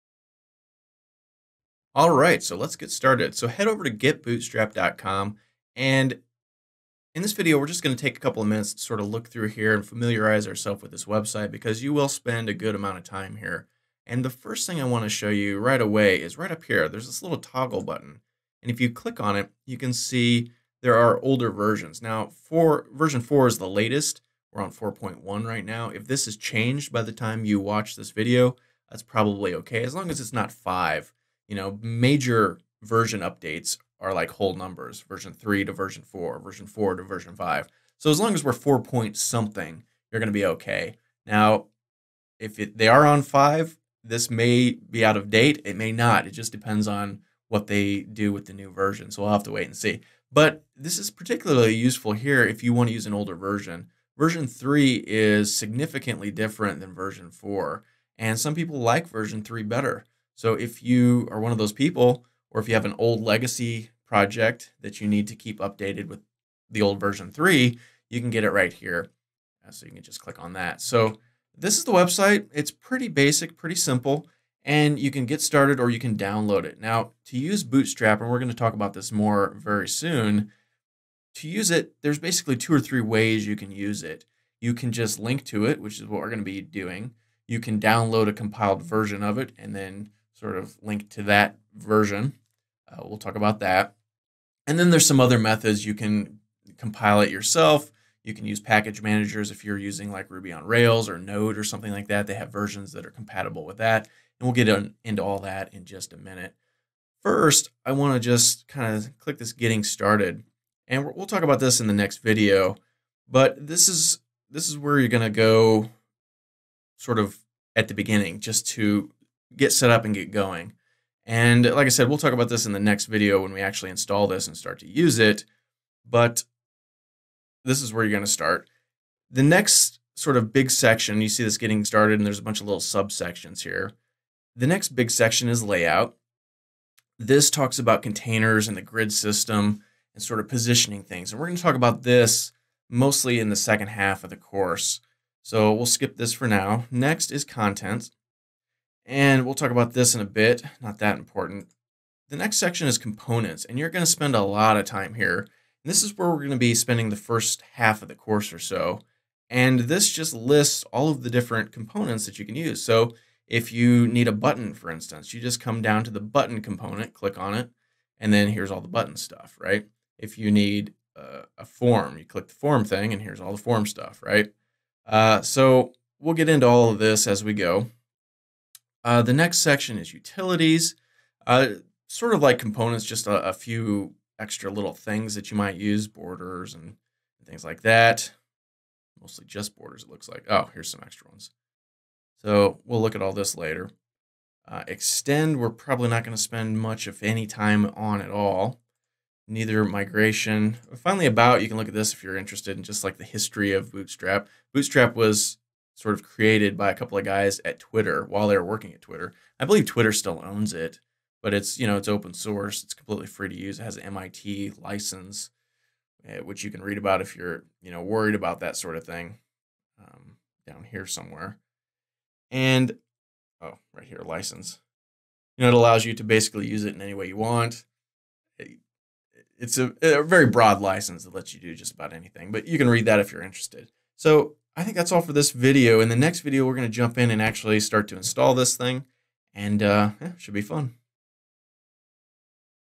Bootstrap 4 For Frontend Web Development - Getbootstrap.com Site Overview
In this course I'll teach you the Bootstrap CSS Framework from beginner to advanced for web development.

In this video I'll walk you through the GetBootstrap.com website and talk about the different sections, and how you'll use them.